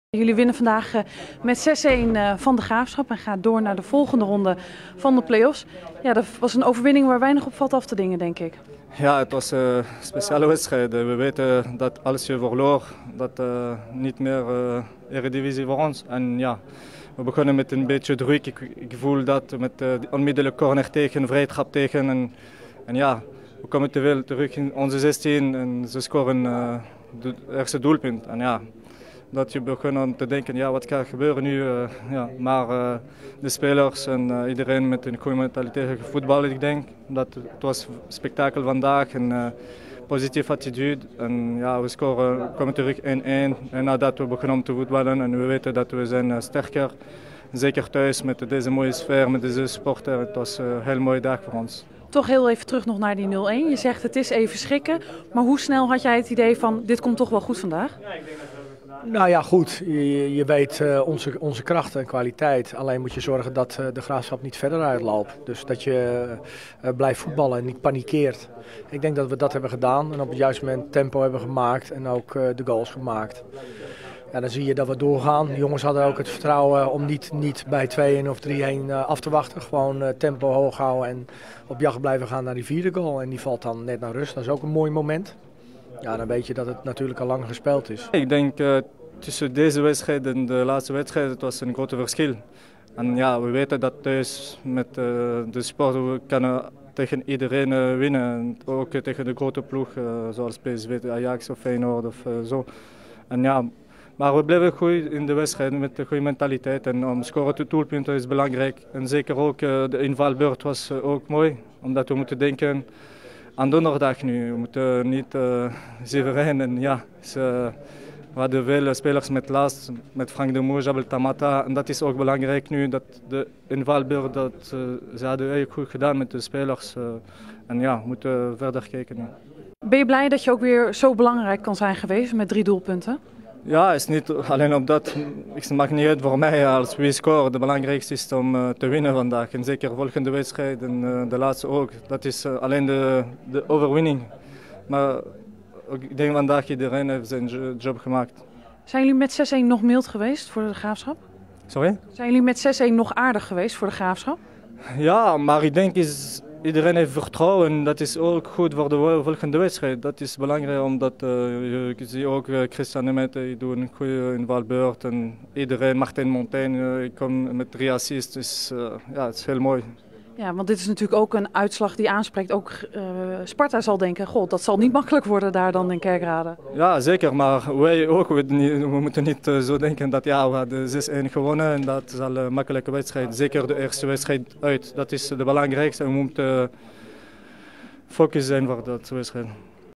Reacties na Roda JC Kerkrade - De Graafschap
Sanharib Malki en Ruud Brood geven een eerste reactie na afloop van het play-off duel tegen De Graafschap. Roda won thuis met 6-1 en speelt nu de finale tegen Sparta Rotterdam.